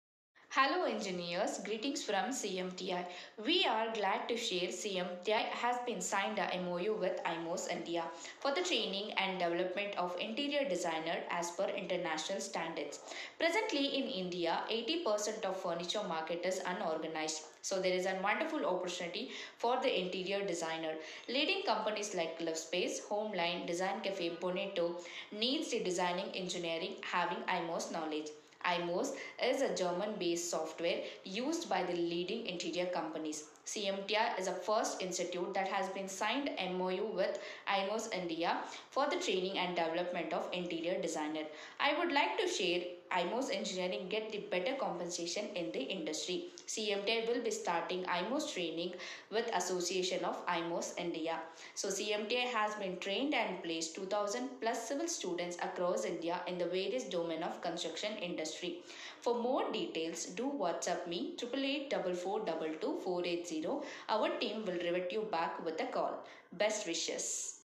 Learn IMOS Software for getting into Interiors Design Field
CMTI in association with Imos India planned to conduct Training Programme on IMOS Software